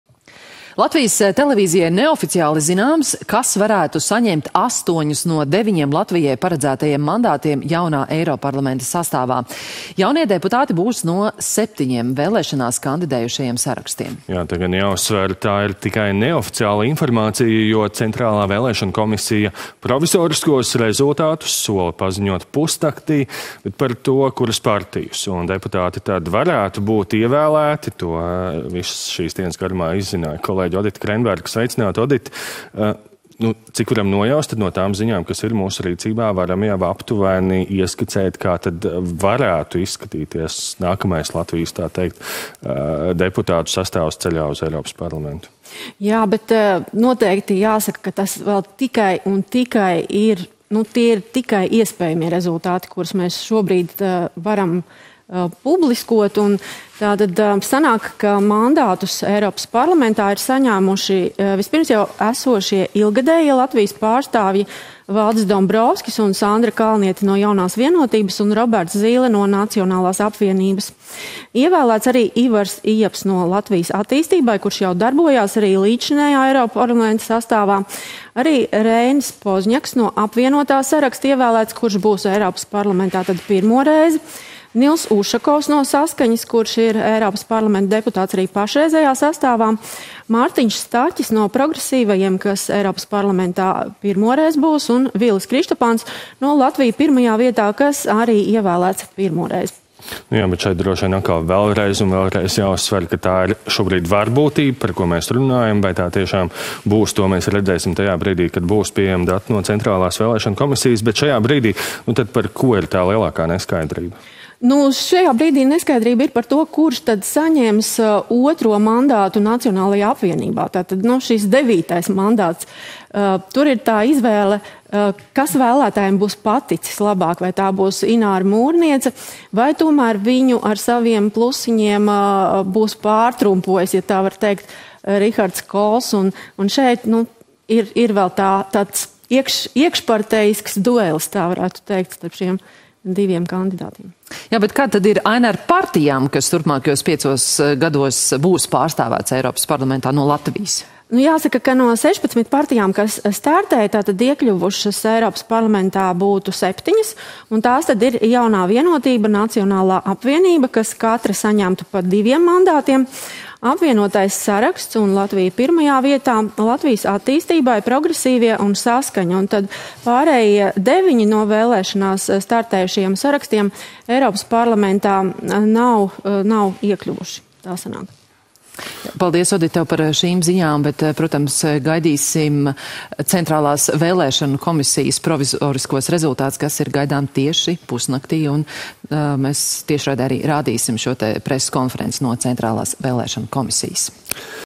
LTV: Pie diviem mandātiem EP tiek JV un NA; iekļūst 7 saraksti
LTV: Neoficiāli zināms, kas varētu saņemt 8 no 9 Latvijai paredzētajiem mandātiem jaunā Eiroparlamenta sastāvā. Jaunie deputāti būs no 7 vēlēšanās kandidējušajiem sarakstiem.

Zināms, ka 9. mandāts būs kādam no Nacionālas apvienības, bet to noteiks "+" un svītrojumu skaits.

Uzsveram – tā gan ir tikai neoficiāla informācija, jo Centrālā vēlēšanu komisija provizoriskos rezultātus sola paziņot pusnaktī.